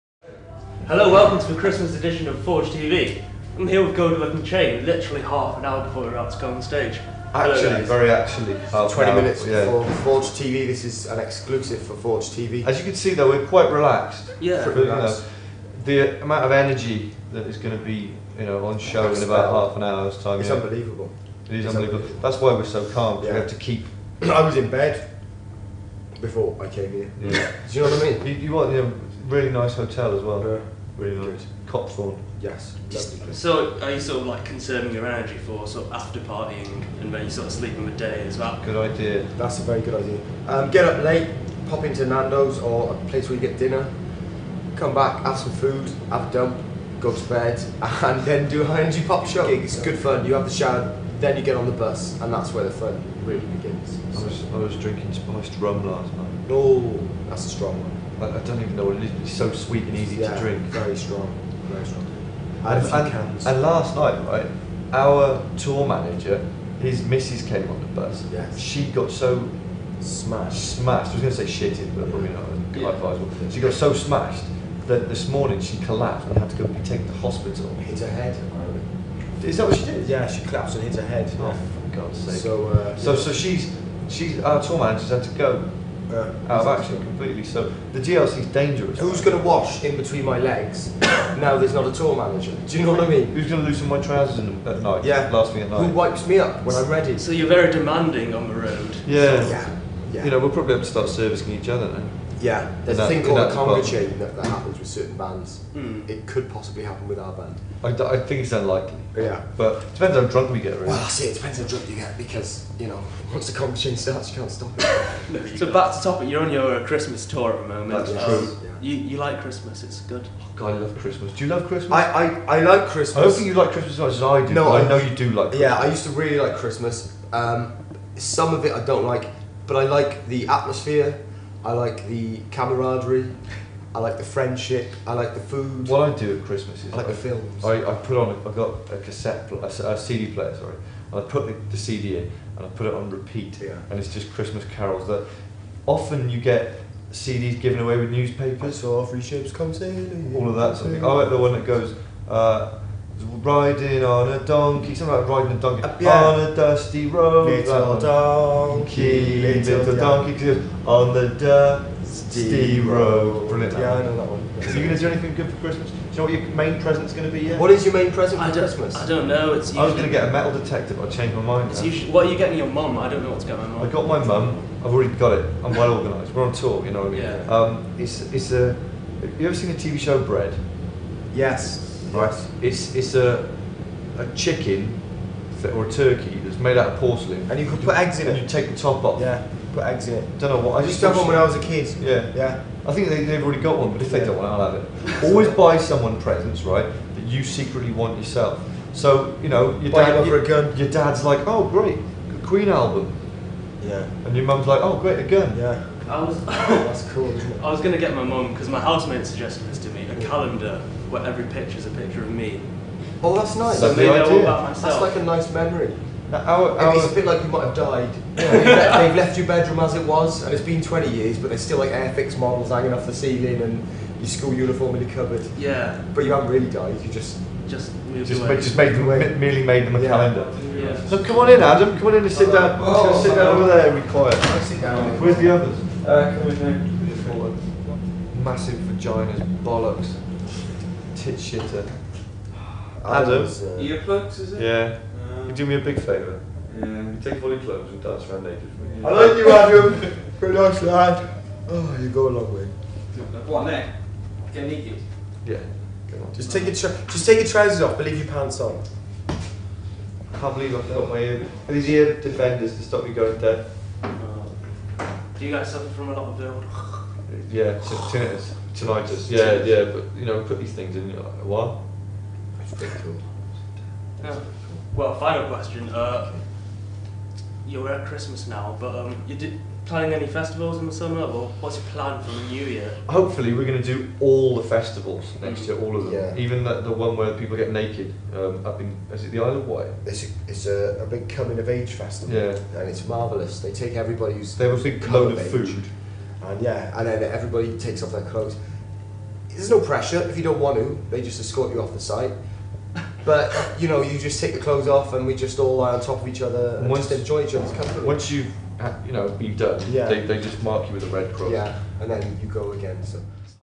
Goldie Lookin Chain
Forge TV interview Goldie Lookin Chain
By Ben Marshall and Nigel Tu